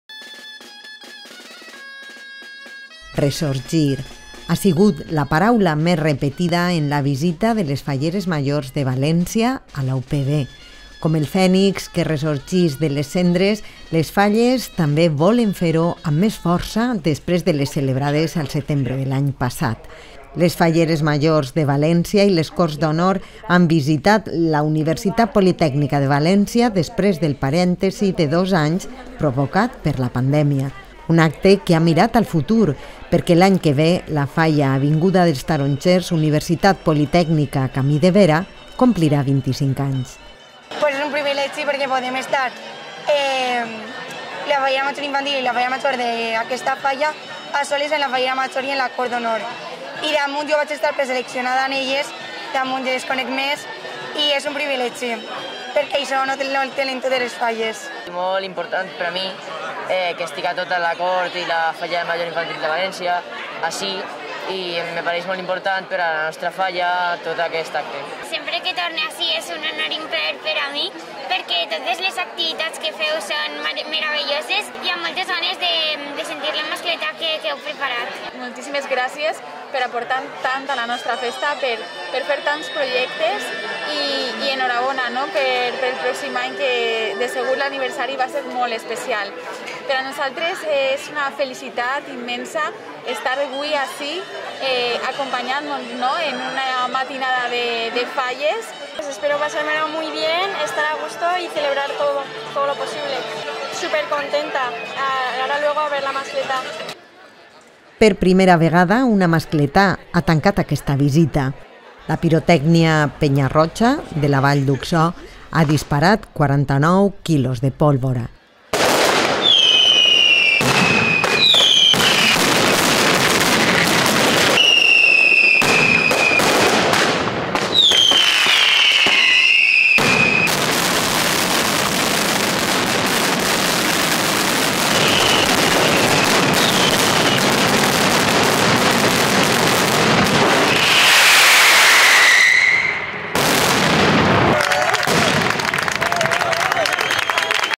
Falleras mayores y mascletà - Noticia @UPVTV, 04-03-2022
Resurgir . Ha sido la palabra más repetida en la visita de las falleras mayores de València a la UPV. Como el ave fènix que resurge de las cenizas, las fallas también quieren hacerlo, con más fuerza, después de las celebradas en septiembre del año pasado.